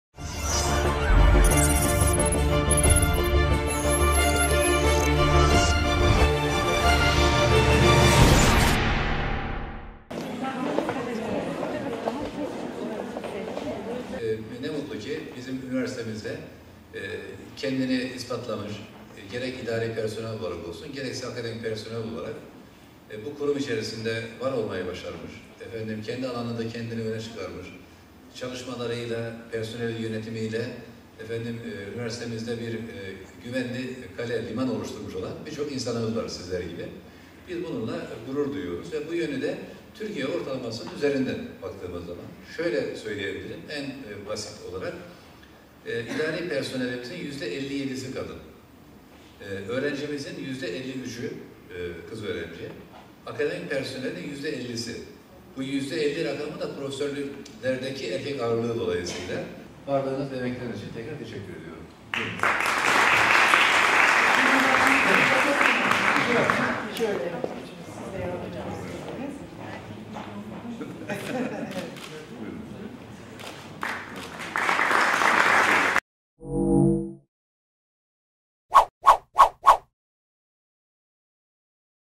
İstanbul Üniversitesi’nin Kadın Mensupları “8 Mart Dünya Kadınlar Günü” Etkinliğinde Buluştu
İÜ Rektörü Prof. Dr. Mahmut Ak, 8 Mart Dünya Kadınlar Günü dolayısıyla düzenlenen etkinlikte İstanbul Üniversitesi’nin Kadın Mensupları ile bir araya geldi.